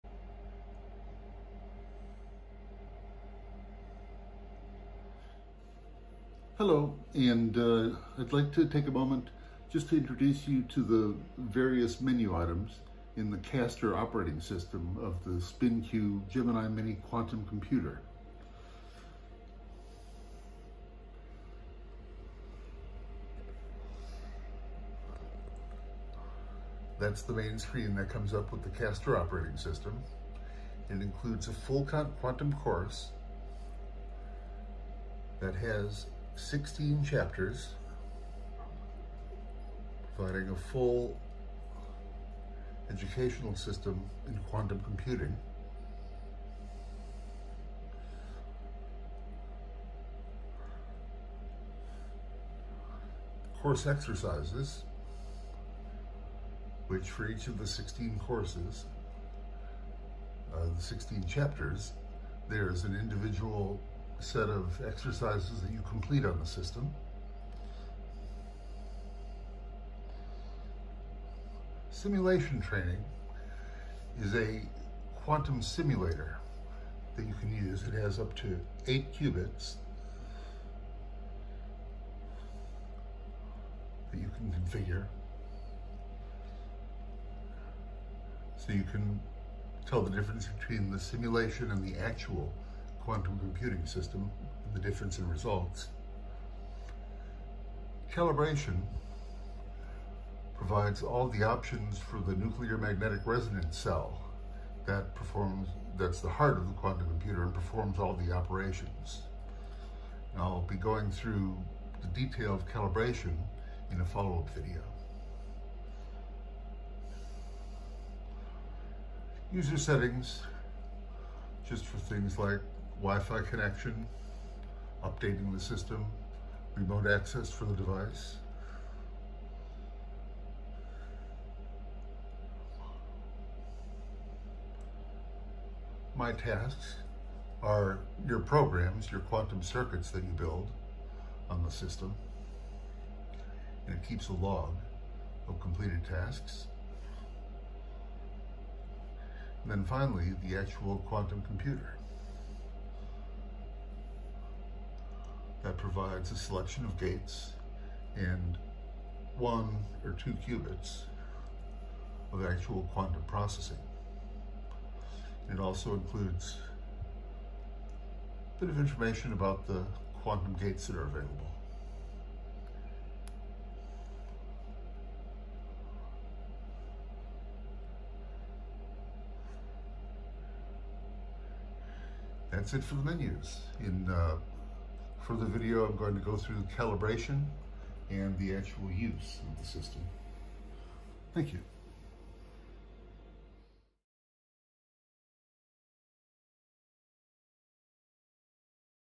SPINQ Gemini Mini Quantum Computer Operating System
This video shows the Castor Operating System of the SPINQ Gemini Mini quantum computer with its built-in quantum computing course, exercises, 8 qubit quantum simulator, calibration, user settings, tasks and 2 qubit real quantum computer.